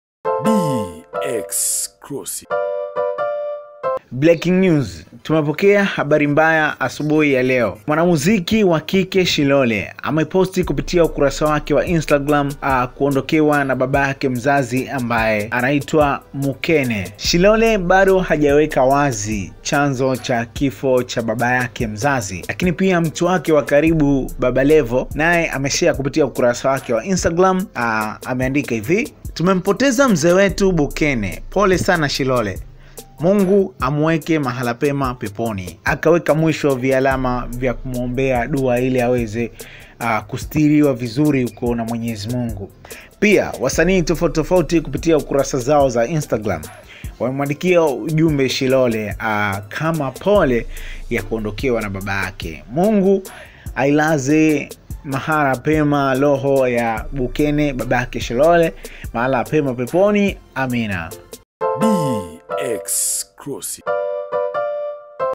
Breaking News : BABA YAKE SHILOLE AFARIKI DUNIA /TAZAMA SHILOLE ALICHOANDIKA
BABA WA MWANAMUZIKI SHILOLE AMEFARIKI DUNIA ASUBUHI YA TAZAMA SHILOLE ALICHOANDIKA KUPITIA UKURASA WAKE WA INSTAGRAM